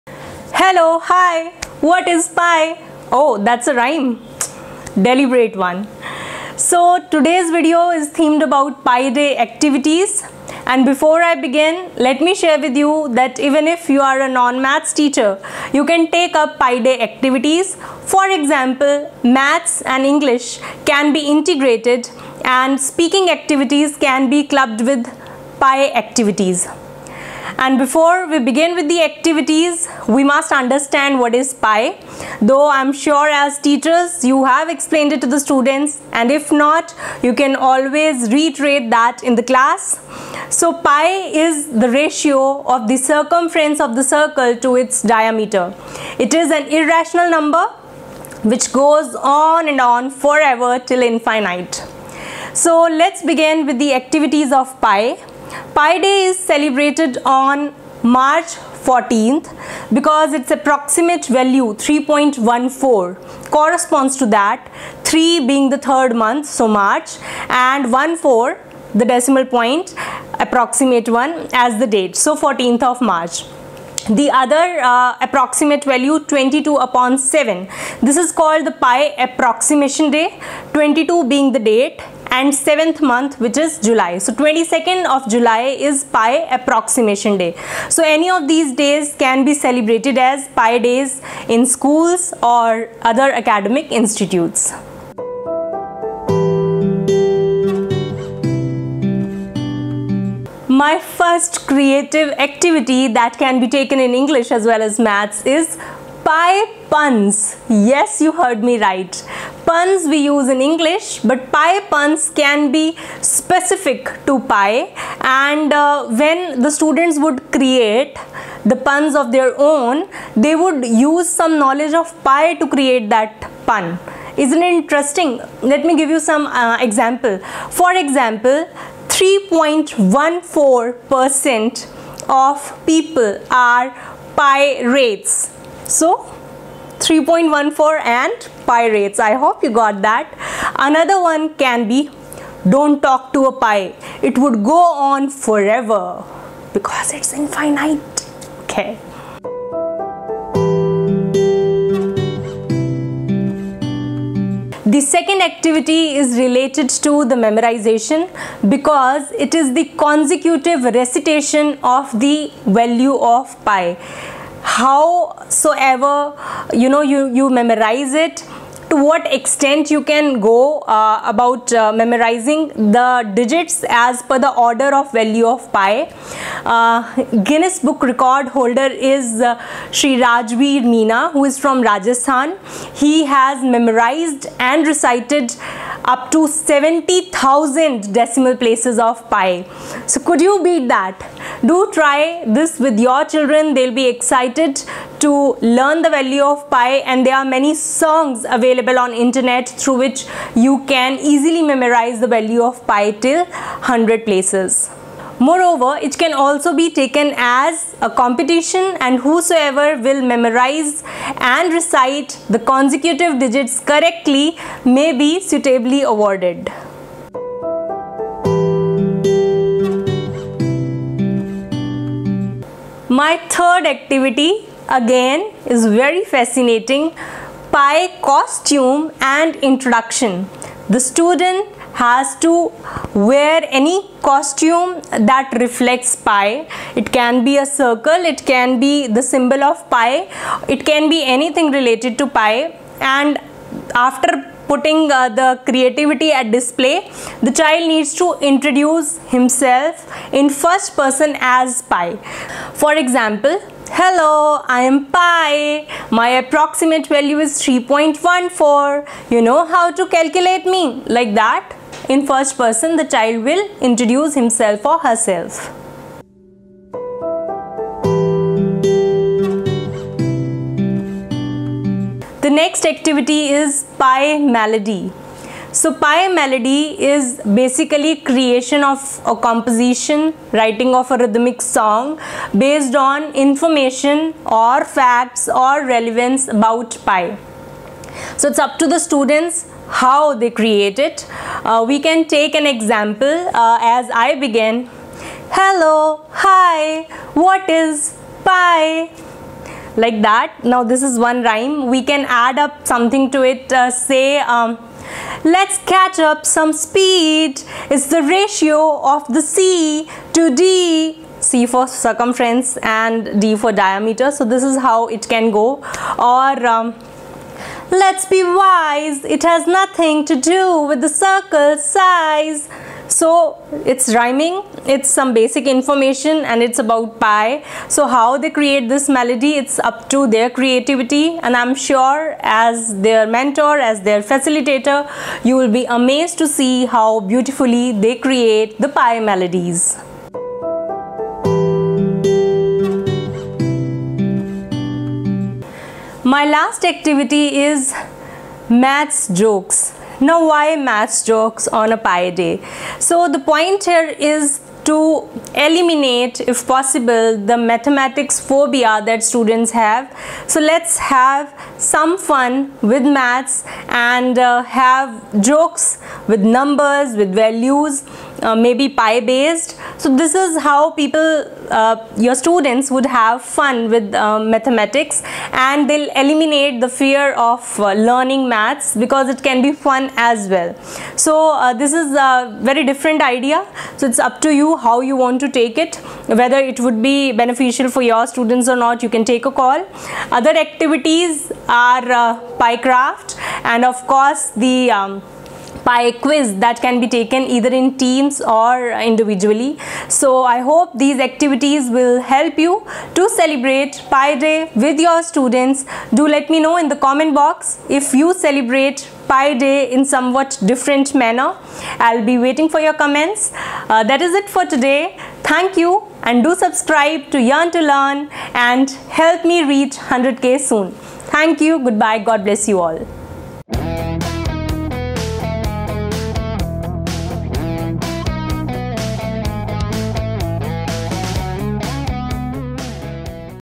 Best Pi Day Activities
Here are amazing activities that you can take on Pi Day either on 14th March or on 22nd July. Pi is an irrational number which is defined by the relationship between the circumference of the circle to it's diameter.